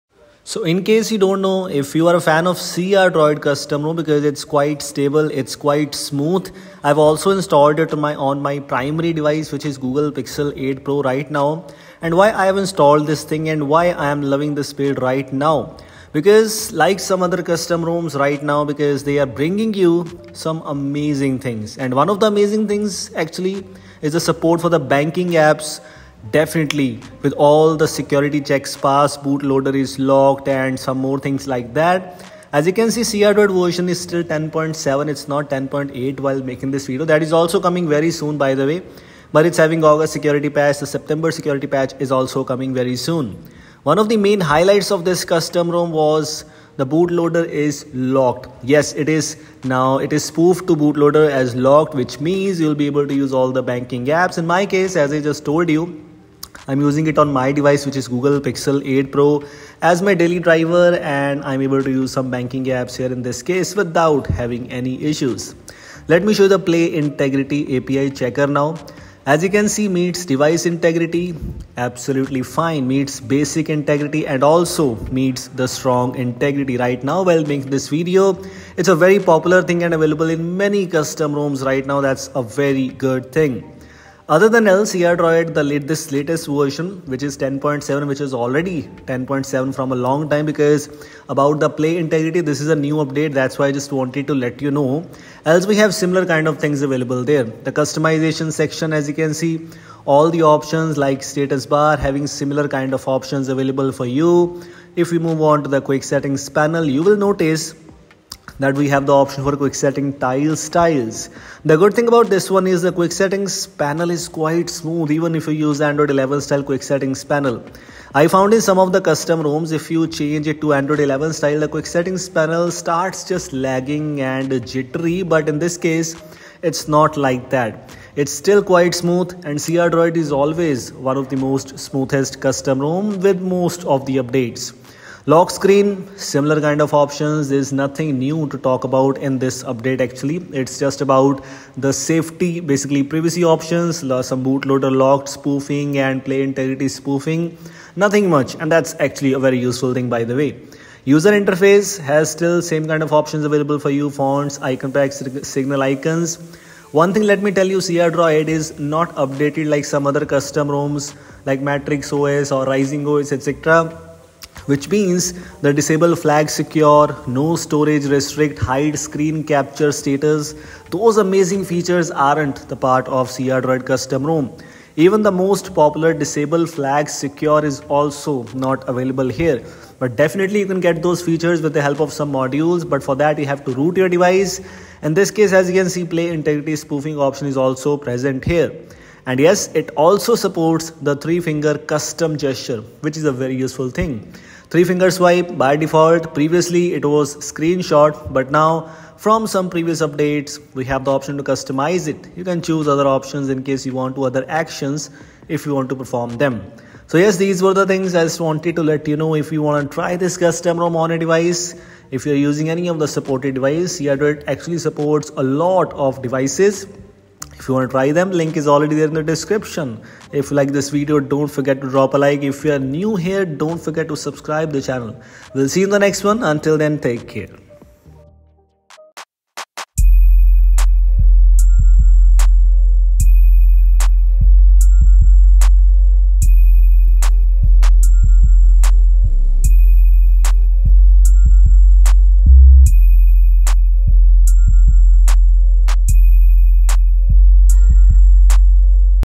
CrDroid is here | Banking Apps working | Bootloader Spoofing Enabled | Finally 🤩
Guess what? CrDroid is finally here! And the best part? It’s got banking apps working, and bootloader spoofing enabled, and it’s all super exciting! 🤩

If you are interested in downloading the ROM, you can find it at the following link:

To access the ROM, you will need to use a VPN if the link is not working.

Best regards,
Karan